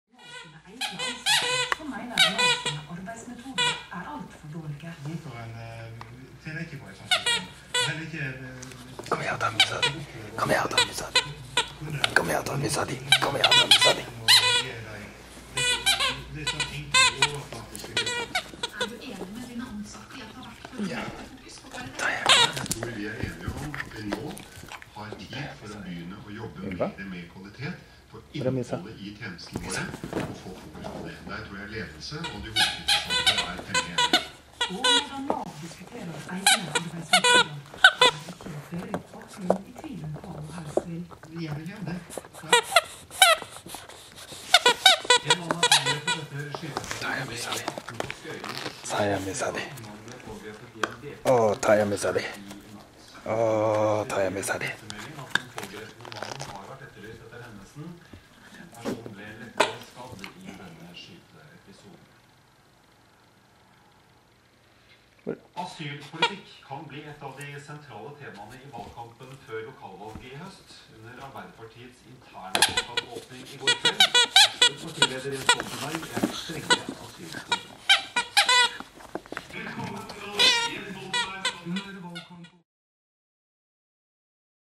Miniature pincher YLVA - and the MOUSE named "MUSA"
Our min pin YLVA is now one year old. She loves to play with her latex mouse named "MUSA". Pip pip!